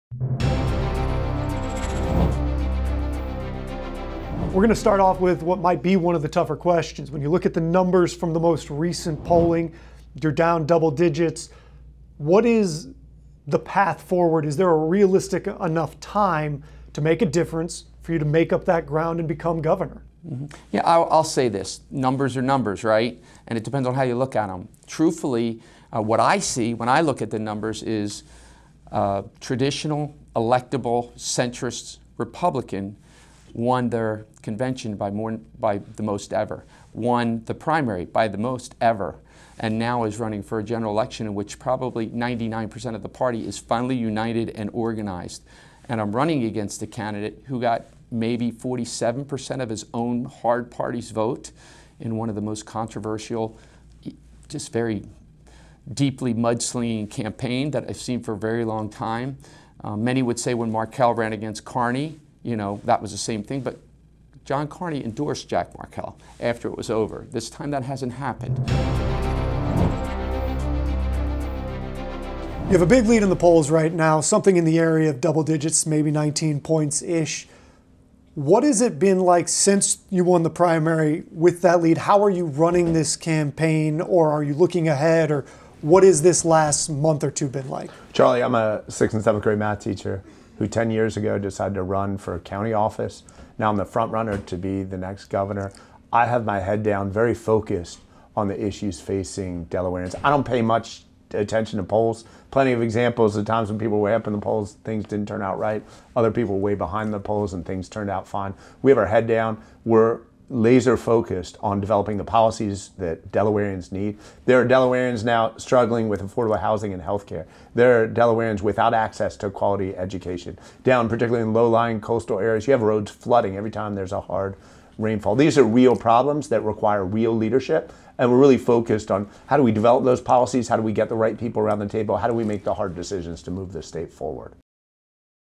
Delaware Gubernatorial Candidates on the State of the Race
The frontrunners for Governor of Delaware share their feelings on the current state of the race, and what they're focusing on as election day draws near.